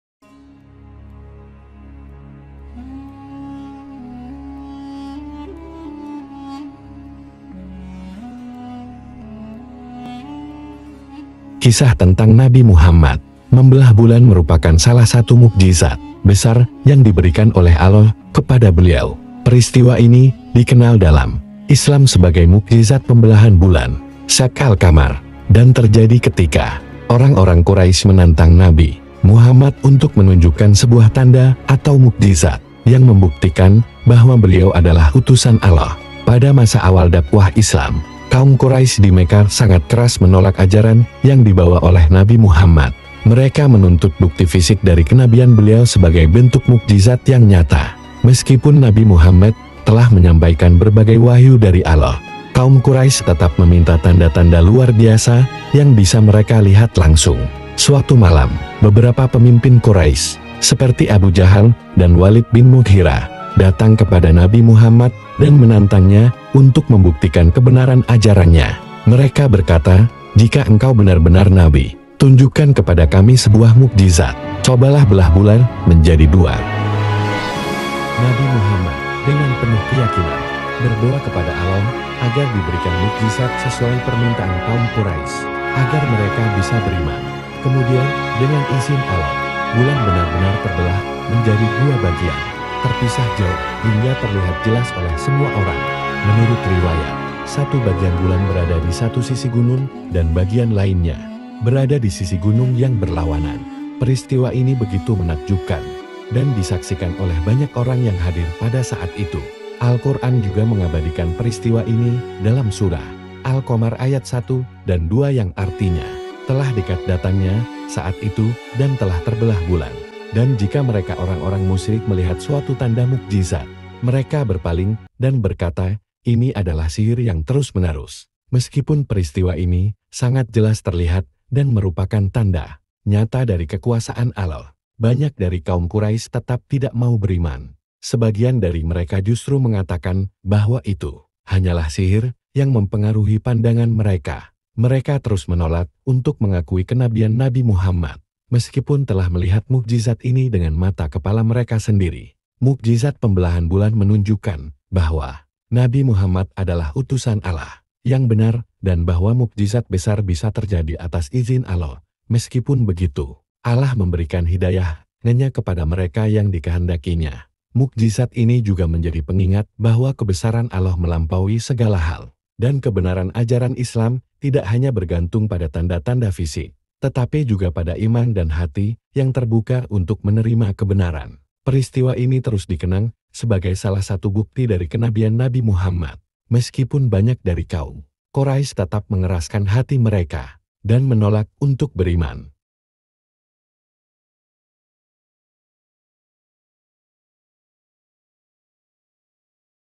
Kisah Nabi Muhammad SAW Dan Mukjizat Pembelahan Bulan
Kisah tentang Nabi Muhammad SAW membelah bulan merupakan salah satu mukjizat besar yang diberikan oleh Allah SWT kepada beliau. Peristiwa ini dikenal dalam Islam sebagai Mukjizat Pembelahan Bulan (Syaqq al-Qamar), dan terjadi ketika orang-orang Quraisy menantang Nabi Muhammad untuk menunjukkan sebuah tanda atau mukjizat yang membuktikan bahwa beliau adalah utusan Allah.

Pada masa awal dakwah Islam, kaum Quraisy di Mekah sangat keras menolak ajaran yang dibawa oleh Nabi Muhammad. Mereka menuntut bukti fisik dari kenabian beliau sebagai bentuk mukjizat yang nyata. Meskipun Nabi Muhammad telah menyampaikan berbagai wahyu dari Allah, kaum Quraisy tetap meminta tanda-tanda luar biasa yang bisa mereka lihat langsung.

Suatu malam, beberapa pemimpin Quraisy, seperti Abu Jahal dan Walid bin Mughirah, datang kepada Nabi Muhammad dan menantangnya untuk membuktikan kebenaran ajarannya. Mereka berkata, "Jika engkau benar-benar nabi, tunjukkan kepada kami sebuah mukjizat. Cobalah belah bulan menjadi dua!"

Nabi Muhammad, dengan penuh keyakinan, berdoa kepada Allah SWT agar diberikan mukjizat sesuai permintaan kaum Quraisy, agar mereka bisa beriman.

Kemudian, dengan izin Allah, bulan benar-benar terbelah menjadi dua bagian, terpisah jauh hingga terlihat jelas oleh semua orang. Menurut riwayat, satu bagian bulan berada di satu sisi gunung dan bagian lainnya berada di sisi gunung yang berlawanan. Peristiwa ini begitu menakjubkan, dan disaksikan oleh banyak orang yang hadir pada saat itu.

Al-Quran juga mengabadikan peristiwa ini dalam surah Al-Qomar (54:1-2)